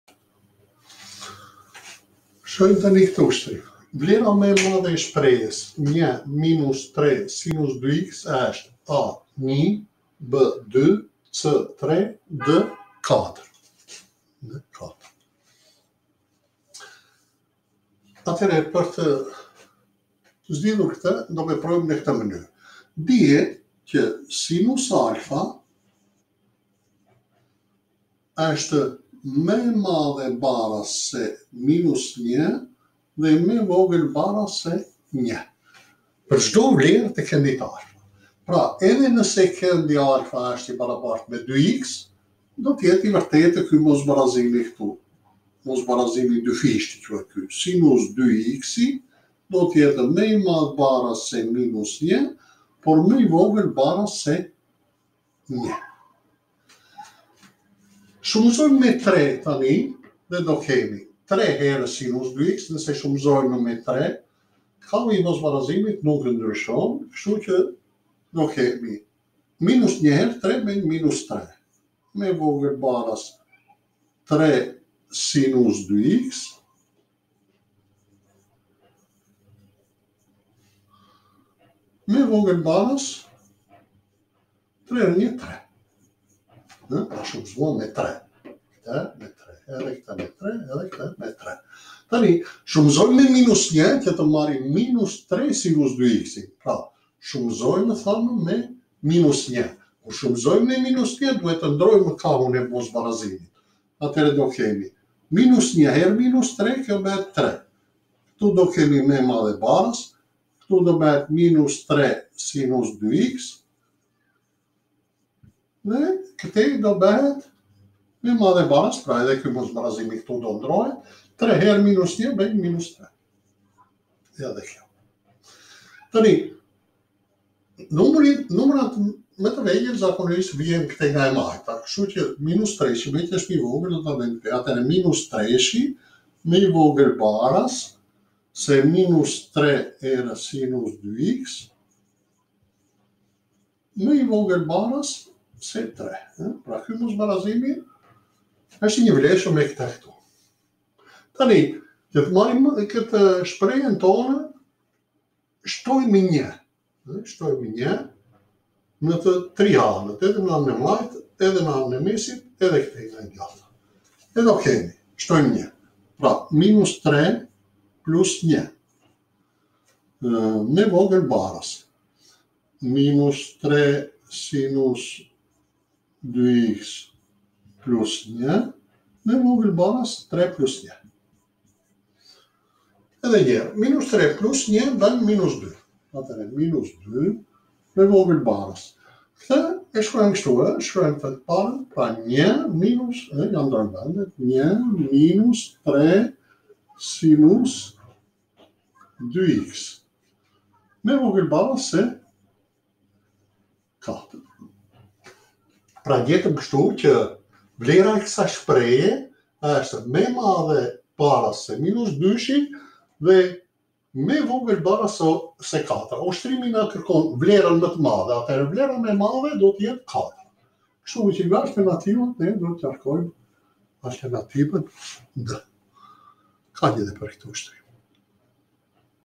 Ushtrime matematike për Maturë /Shprehje që përmban sin2x.
Në këtë seri videosh do të zgjidhim disa ushtirime nga matematika. Do të ndihmohen nxënësit por edhe ata që janë dashamirës të matematikës. Meqenëse provimet e matures janë së afërmi, do të fillojmë me disa ushtrime që i përshtaten ushtrimeve të shkollës së mesme. Në grupin e pare, do të zhvillojmë disa ushtrime që kanë të bëjnë me kuptimin për bashkësinë, prerja dhe bashkimi i bashkësive, bashkësia e numrave reale, kuptimi i fuqisë me eksponentë realë, veprime me fuqi të tjera.
Gjithashtu do të japim edhe disa kuptime mbi logaritimin e një numri, kuptimin e rrënjës, dhe ndonjë ushtrim nga logjika matematike.